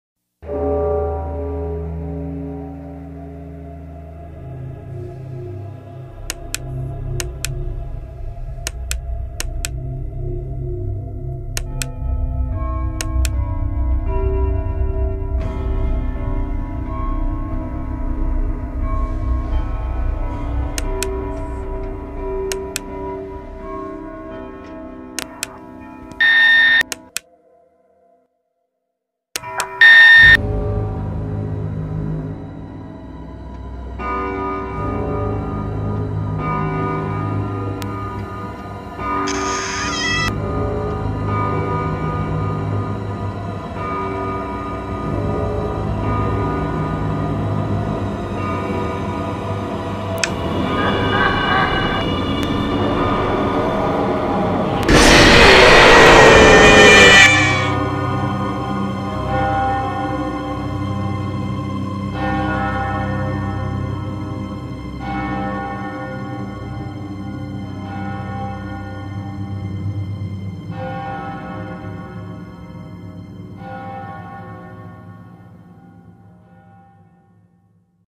SFM Five Nights at Freddy's and Equestria Girls Tribute 4 Trailer 2
The second Trailer of SFM Five Nights at Freddy's and Equestria Girls Tribute 4

Coming to June 2nd